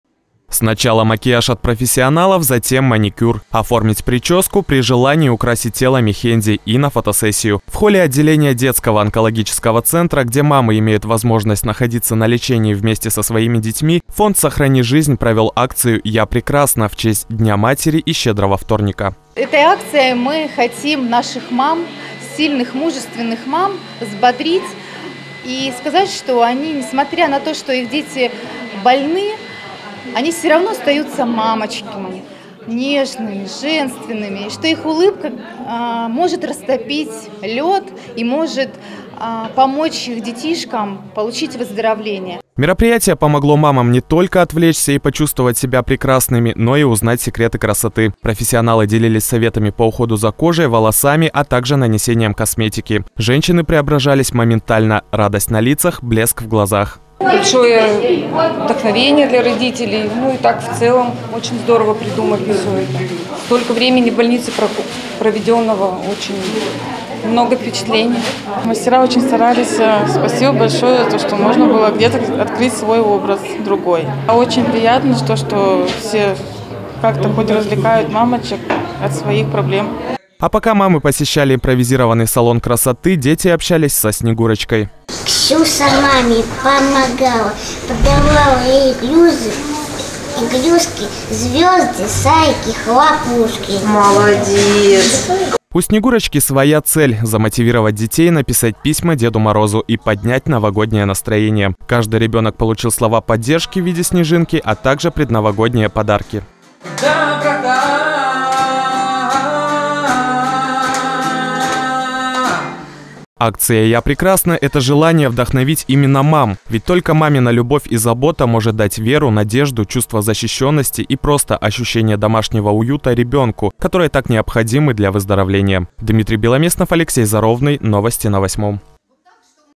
ВДОХНОВЕНИЕ ДЛЯ МАМЫ
В рамках международной акции «Щедрый вторник» и Дня матери, благотворительный фонд «Сохрани жизнь» проводит ряд мероприятий. Так, во Владивостоке, в краевом детском  онкологическим  центре прошла  акция под  названием «Я прекрасна». Мамы тех детей, которые проходят лечение в центре,  могли  пообщаться с профессионалами из сферы красоты и здоровья.